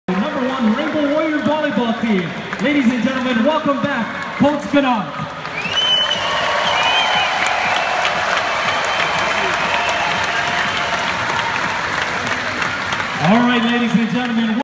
Hawaii men's basketball coach Eran Ganot introduced
Eran Ganot, new coach of the University of Hawaii men's basketball team, was introduced before a Rainbow Warriors volleyball match.